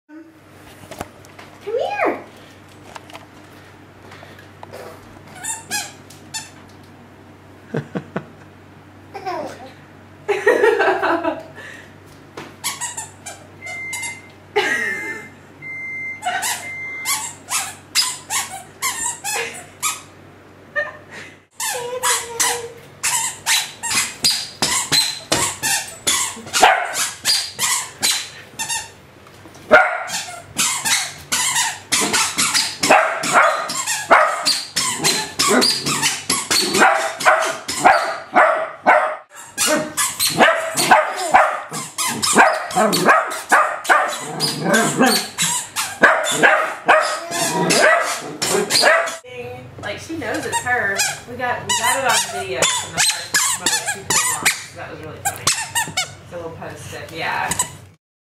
squeaker shoes
miriam gets new squeaker shoes